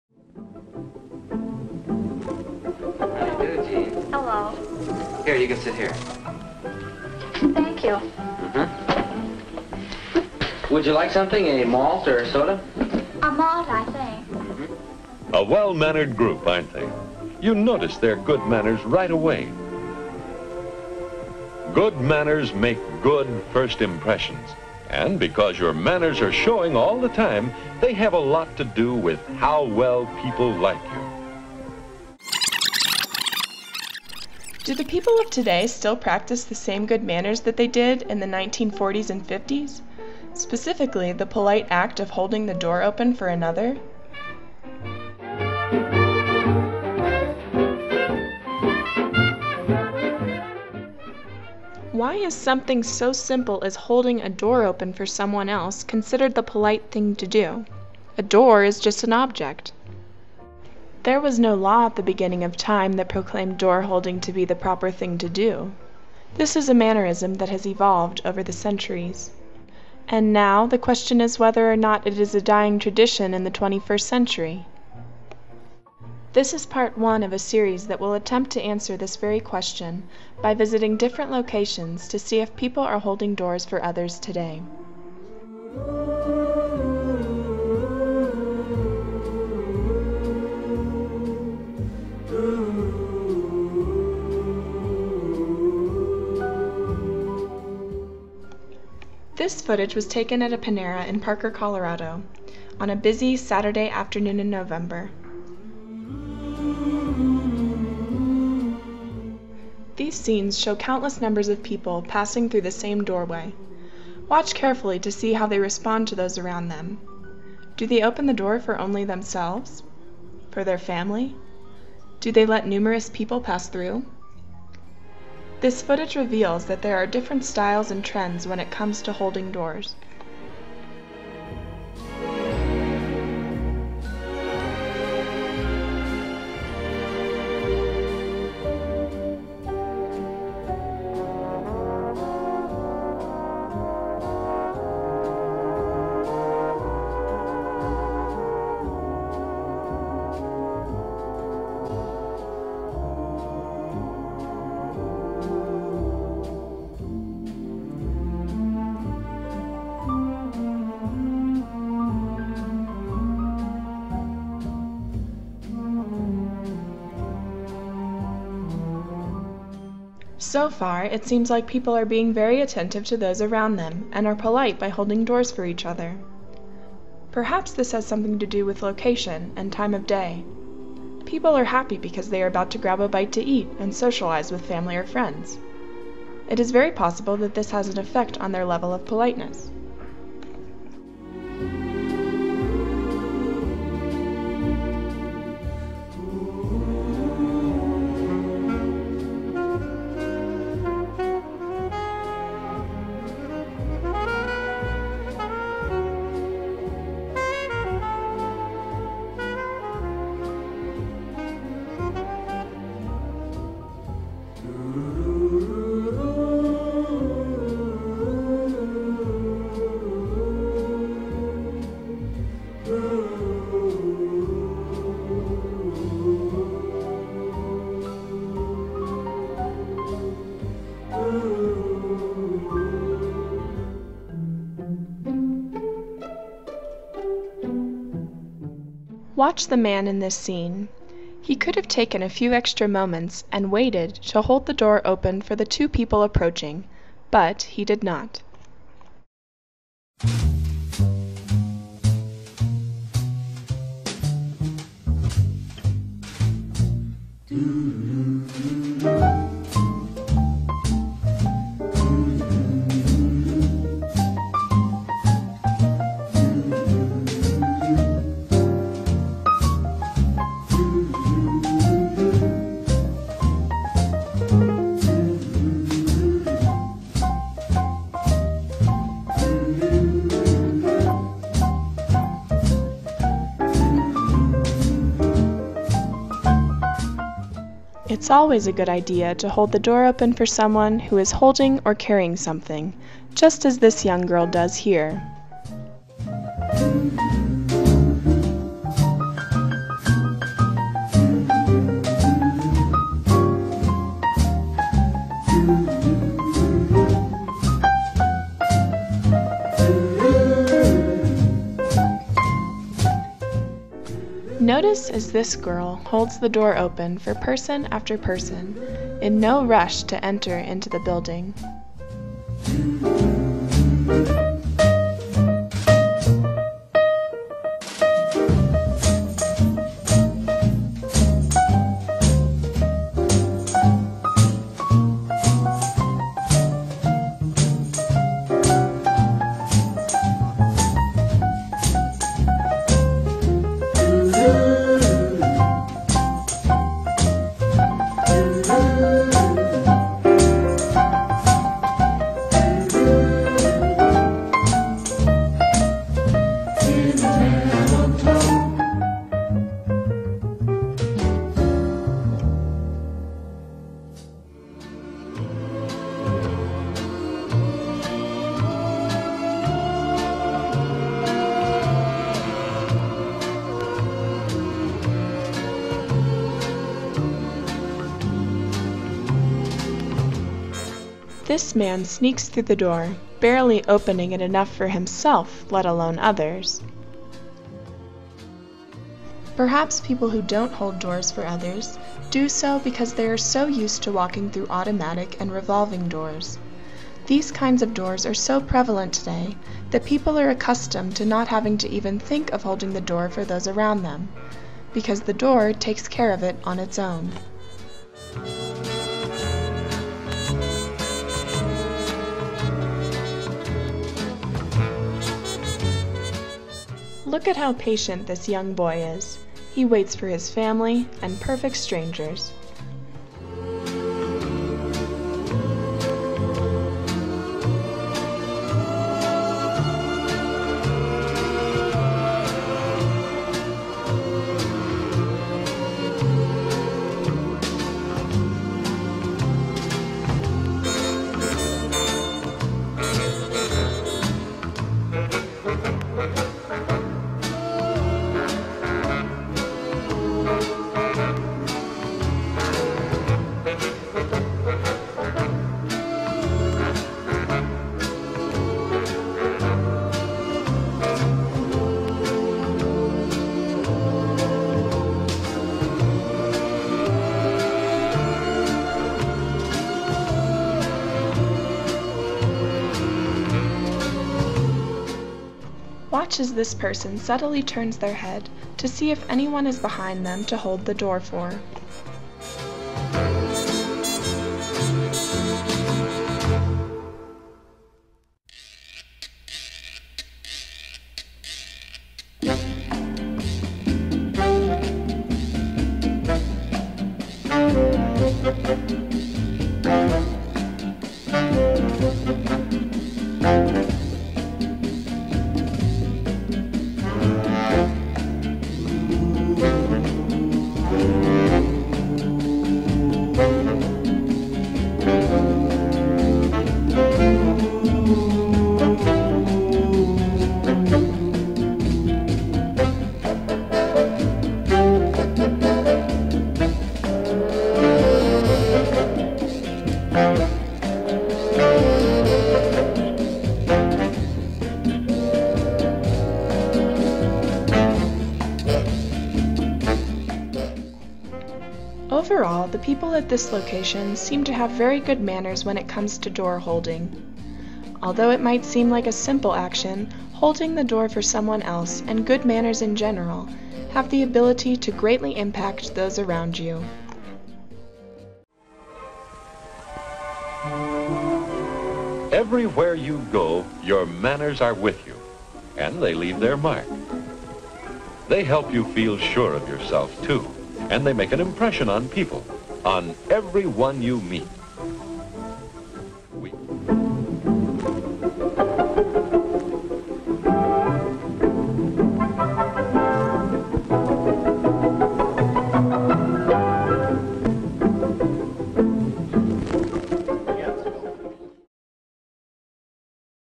Fix It - Door Holding
I have created this short documentary to heighten people's awareness of the importance of holding doors for others.  With this video I want to make others more mindful of those around them when they are passing through a doorway.  From my own personal experiences, I have noticed that in general, the tradition of door holding is beginning to fade away and people are becoming less aware of those around them.
 This piece attempts to answer the question "Is door holding a dying tradition?' In this video I compare door holding of the mid-twentieth century with today.  My goal is to determine whether, in general, the people of today hold doors for others or if, as a society, we have become careless in this area.
 I have mixed movie clips from the mid-twentieth century with my own modern day footage.  Today, the 1940s and 50s are characterized as a time when etiquette and good manners were at their peak.  In comparing my own footage to these old clips one can see how the tradition of door holding has evolved over the decades.
 To some, holding the door for another might seem like a meaningless act.  Regardless if it's making a large impact on the life of the one that we hold the door for, it is an act of consideration for one's fellow man.   We must be considerate to one another in small ways if we hope to resolve more serious issues.